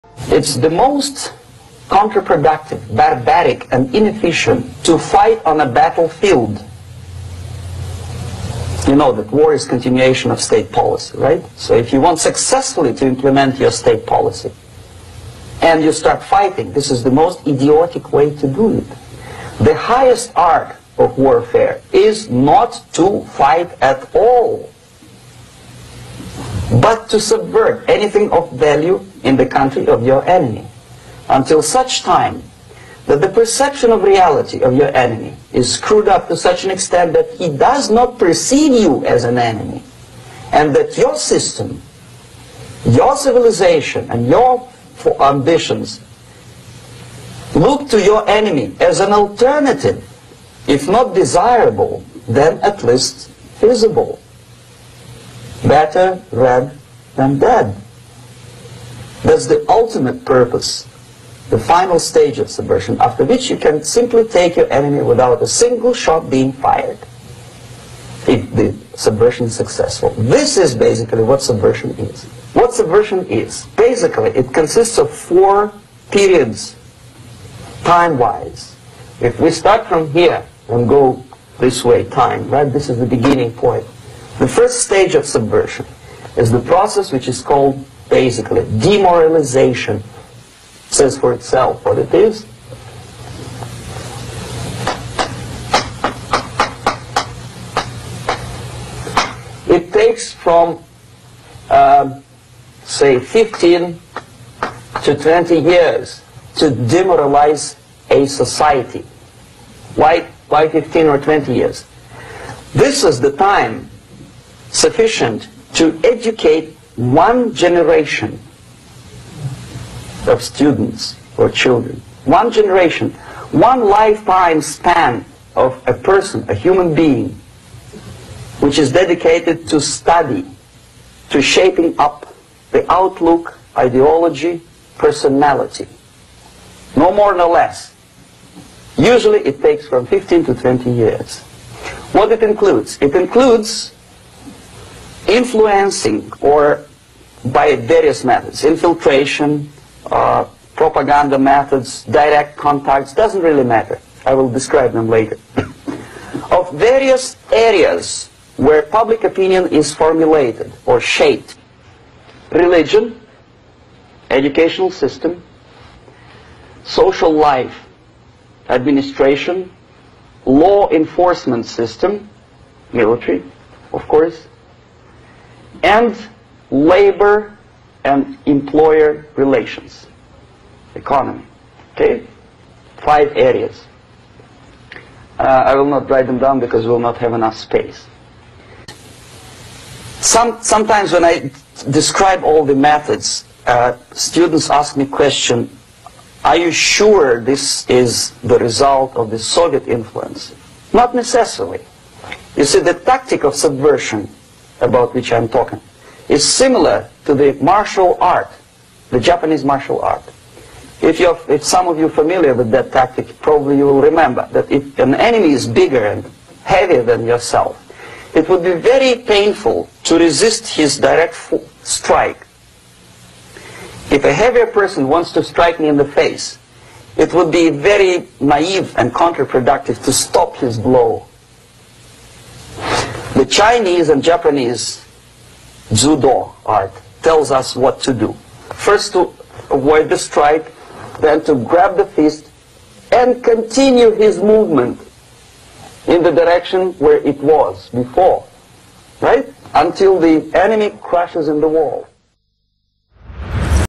عميل جهاز المخابرات السوفيتي السابق: أعلى فنون الحرب للقضاء على أي دولة هو عدم القتال على الإطلاق
توماس شومان عميل جهاز المخابرات السوفيتي السابق يوضح كيف تدمر دولة دون طلقة واحدة  | 29 أكتوبر 2018